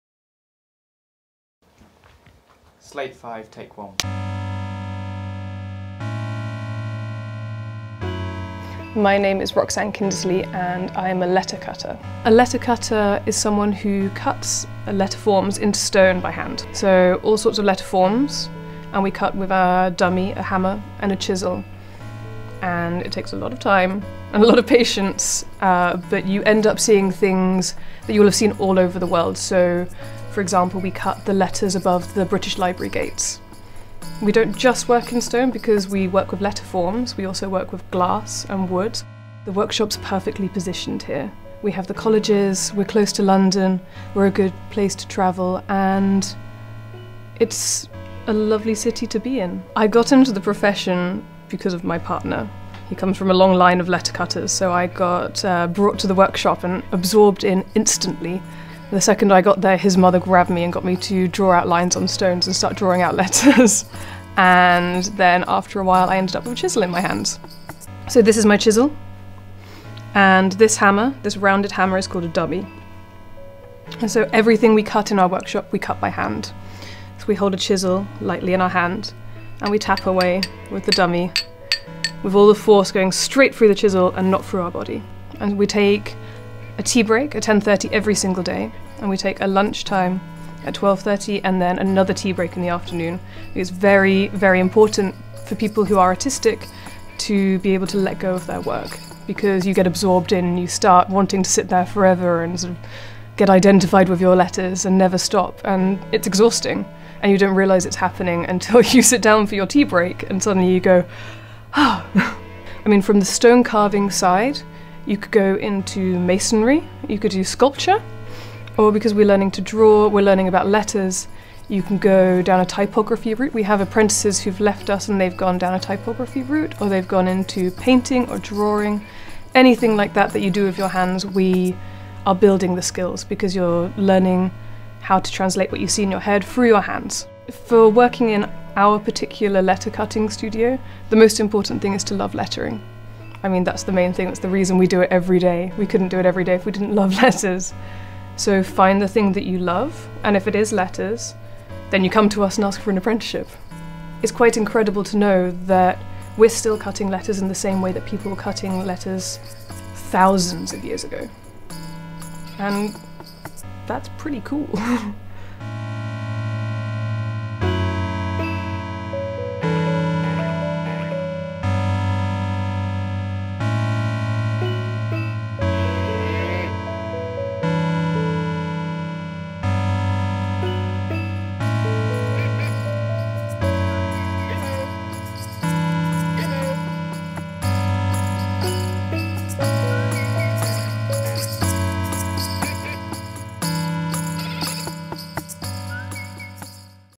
Roxanne Kindersley | Letter cutter
This video was made by Reel Connections CIC with students from Soham Village College as part of Festival Bridge's Creating The Future project. Creating The Future aims to build effective information, advice and guidance to make the creative industries a viable career pathway for young people.